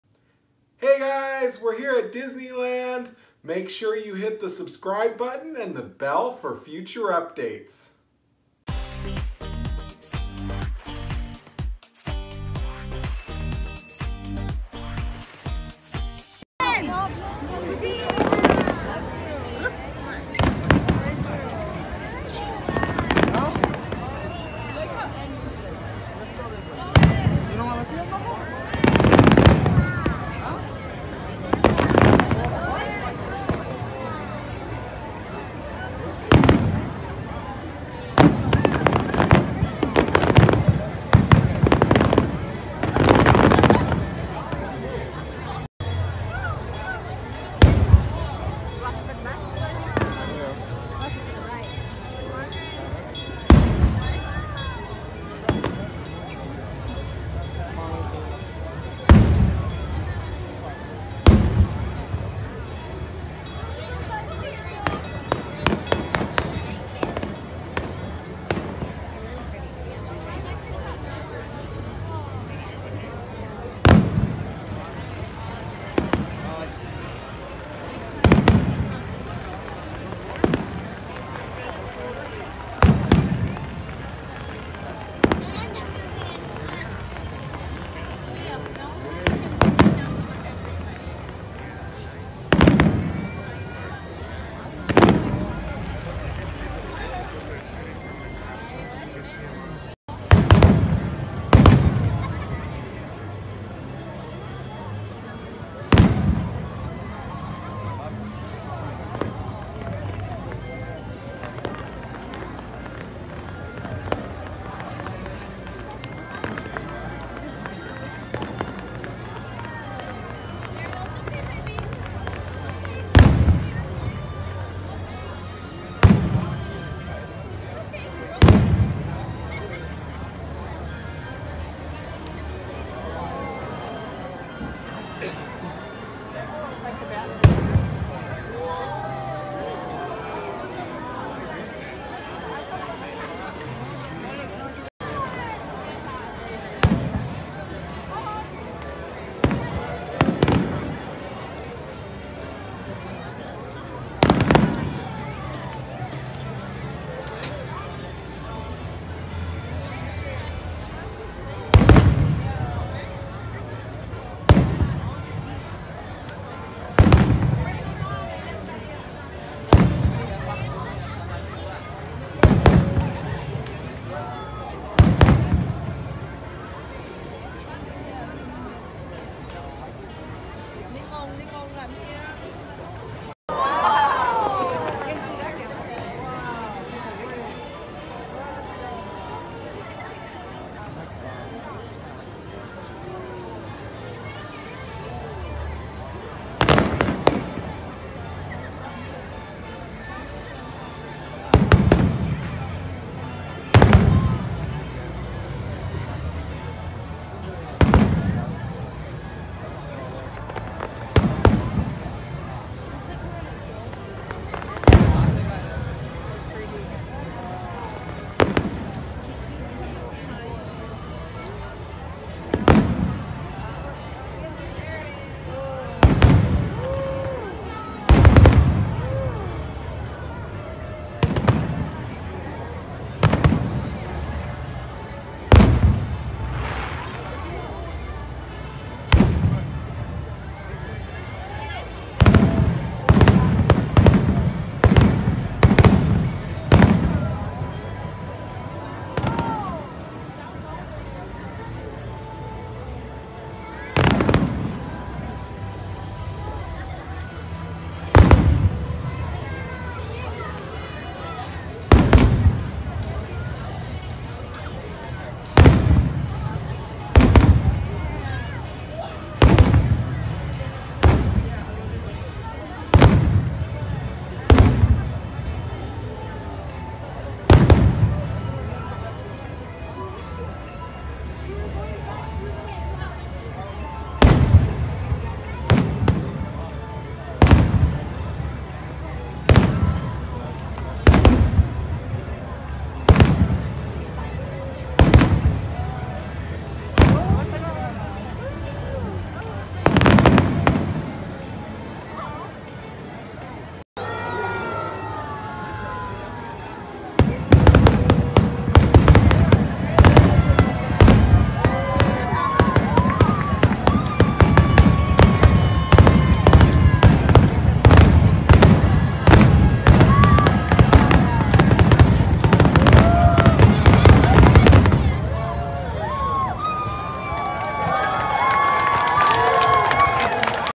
Disneyland Fireworks June 2018 - Outside Parks
As we were walking out of Disney California Adventure last night we got to see the fireworks from in between the parks. This wasn't the most optimal viewing but it was awesome to still be able to see the fireworks and the train station.